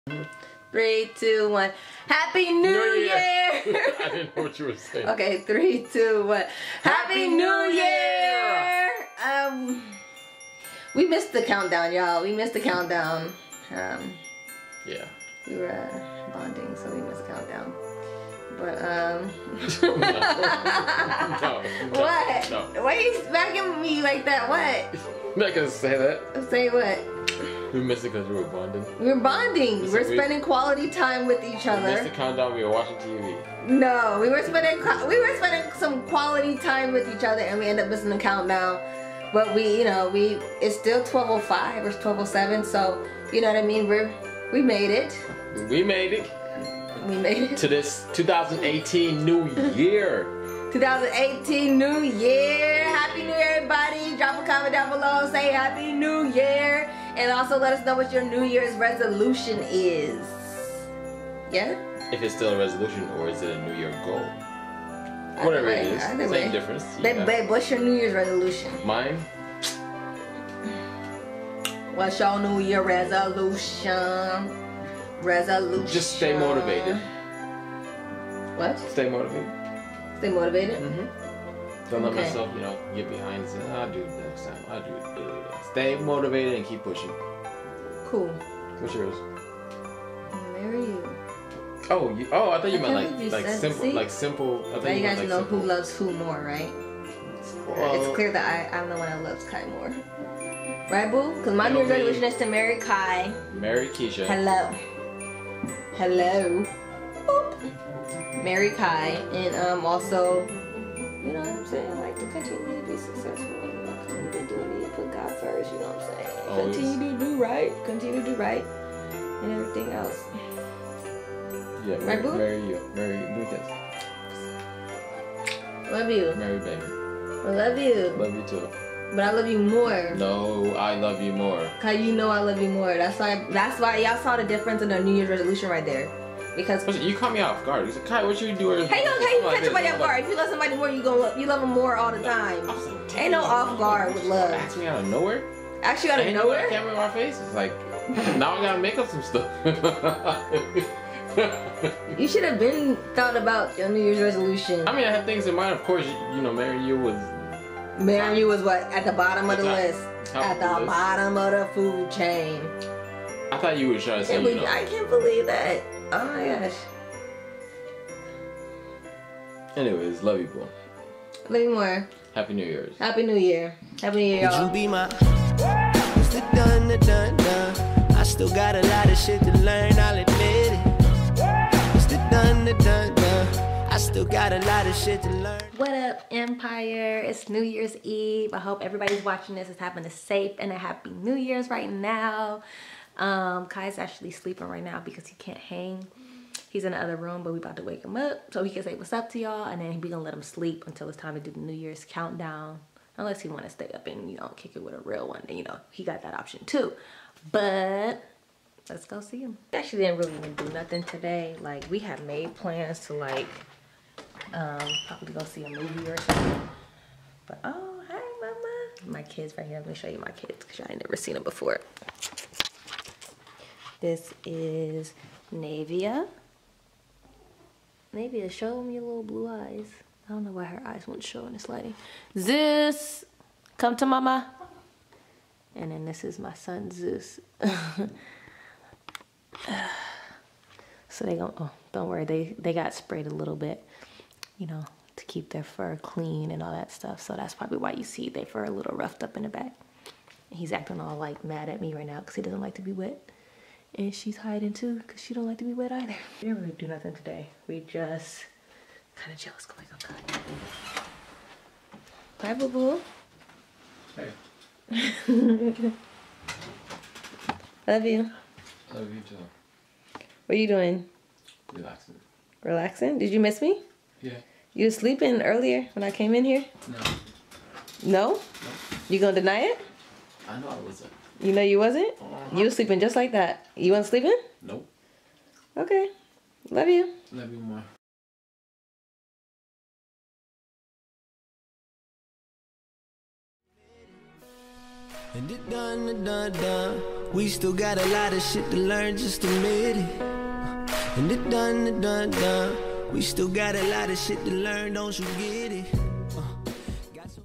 VLOG| HAPPY NEW YEAR!!! 2018 2K18 | NEW YEAR'S RESOLUTION | NEW YEAR'S EVE
What up EMPIRE!!! Soo I literally rode into the new year and ended up missing the countdown LOL, but we wanted to share our 2018  new year's resolution with you all! WE APPRECIATE EACH AND EVERY ONE OF YOU!!